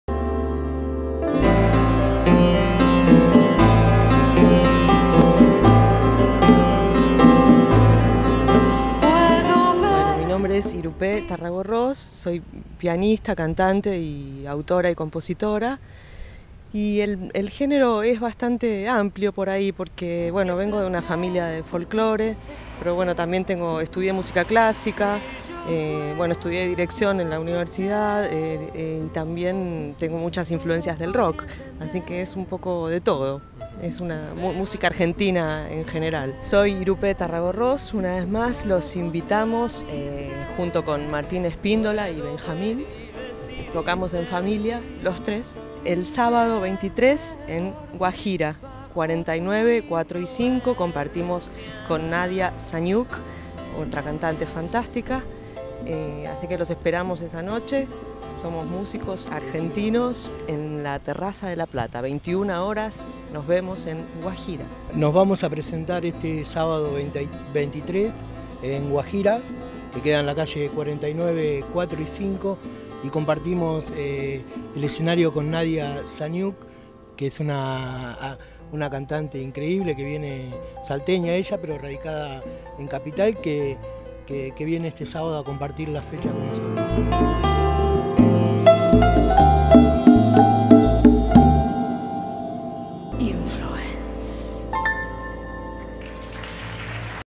CTVNoticias | IRUPÉ EN GUAJIRA
Se presentará este sábado 23 en 49 e/ 4 y 5 junto a Nadia Szachniuk. Una velada imperdible llena de música argentina a partir de las 21 hs.